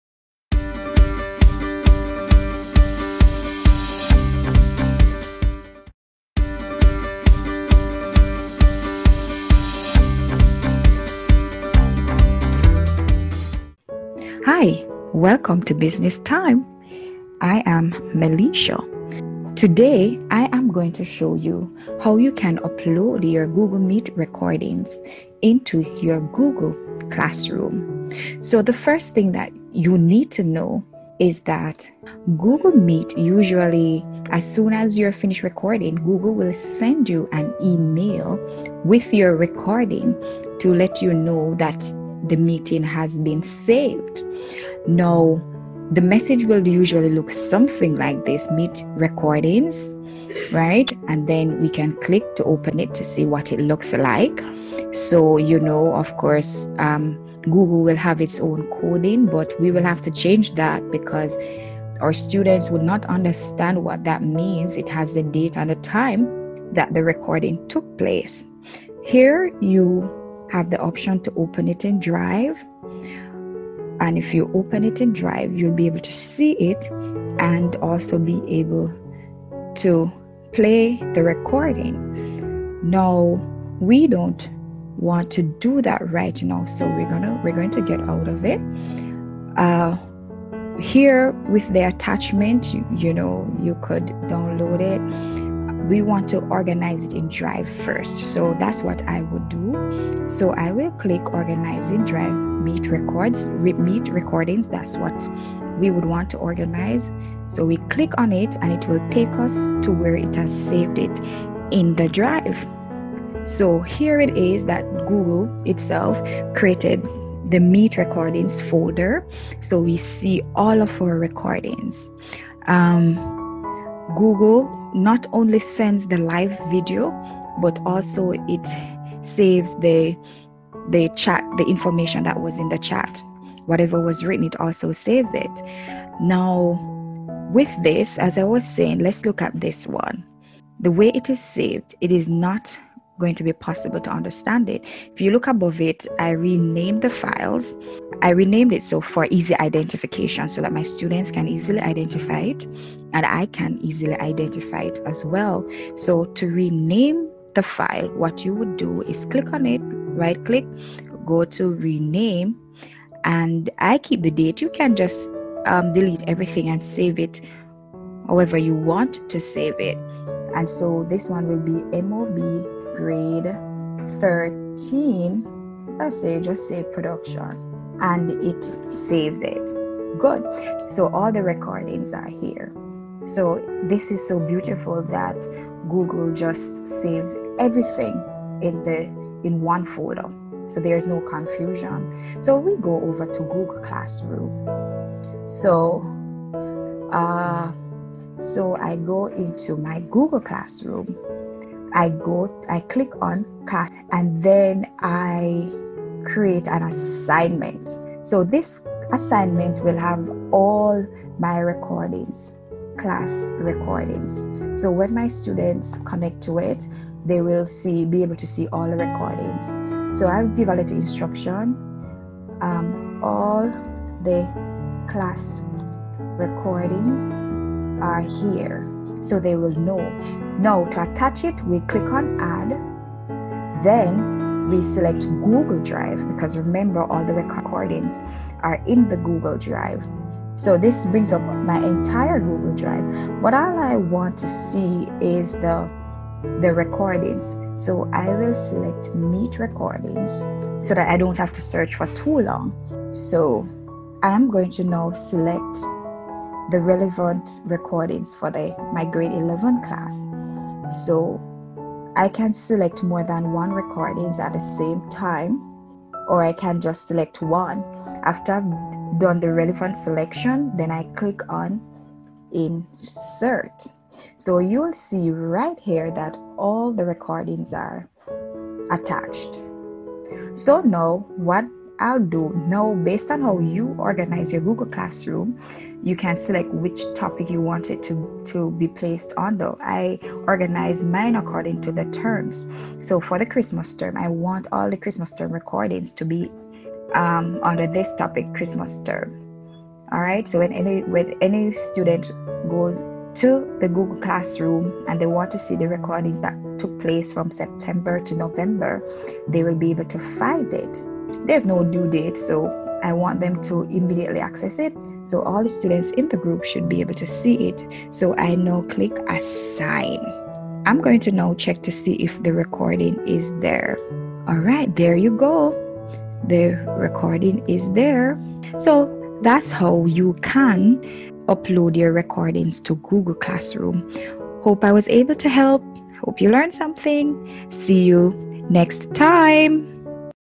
How to upload Google Meet Recordings in Google Classroom
This video demonstrates how you can upload your Google Meet recordings to your Google Classroom.